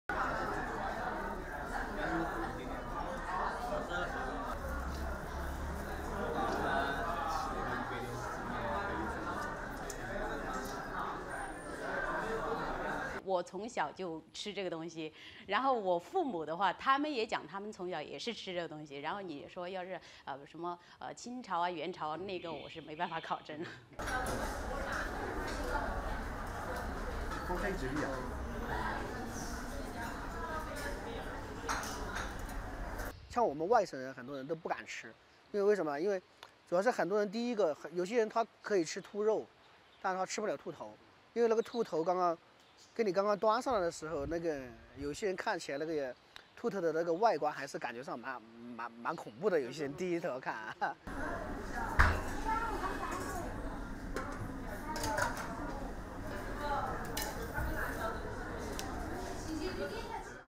Spicy marinated rabbit heads -- a hit in China's Sichuan
The Chinese province of Sichuan is renowned for its spicy, peppery local dishes: one of its favourites is rabbit brains. The popularity of the animal has forced supplies to be imported from France.